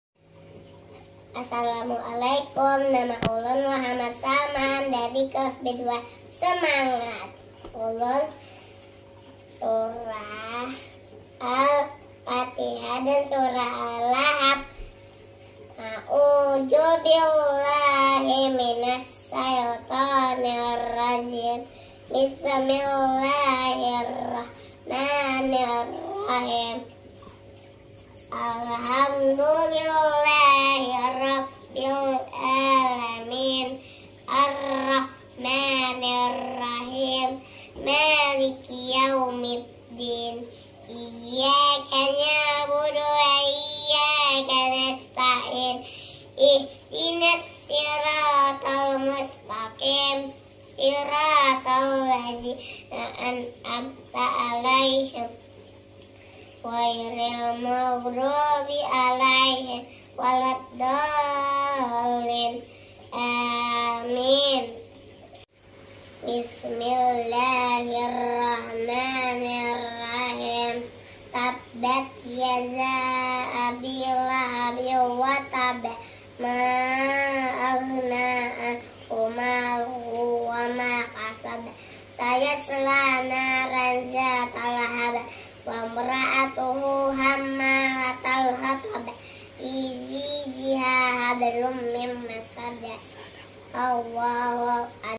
Muhammad Saman (Lomba Menghafal Surah Al-Qur'an) | Semarak Pekan Muharram 1444 H
Nama : Muhammad Saman
Kelas : TK-B2 Semangat

Dalam rangka menyambut *Tahun Baru Islam 1444 H*, PAUD IT Ihsanul Amal mengadakan kegiatan berbagai macam perlombaan video yang bisa ananda ikuti 🤗🤗🤗

🕌 *Semarak Pekan Muharram PAUD IT Ihsanul Amal* 🕌
- *Jenjang KB : Lomba Video Membacakan Cerita Untuk Ananda (Cerita dibacakan oleh Ayah/Bunda)*
- *Jenjang TK-A : Lomba Video Nasyid*
- *Jenjang TK-B : Lomba Video Menghafal Surah Al-Qur'an*

☪️🕌 Yuk mari bersama menggapai berkah di bulan Muharram 1444 H ☪️🕌